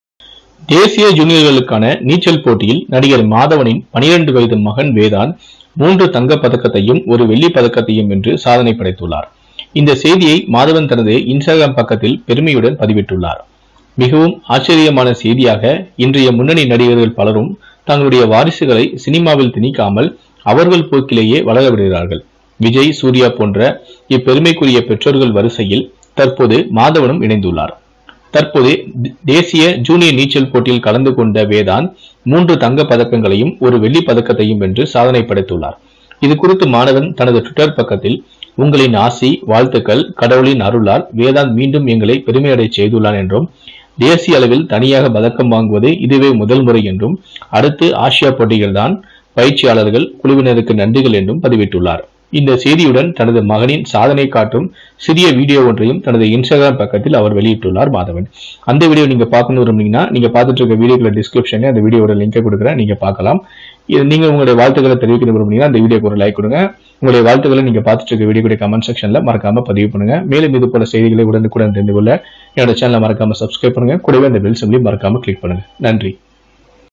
அனந்த கண்ணீரில் மகனை நினைத்து கண்கலங்கிய மாதவன் | Actor R Madhavan son Vedaant
Actor R. Madhavan son wins 3 golds and a silver at the Junior Nationals Swim meet at Junior National Aquatic Championships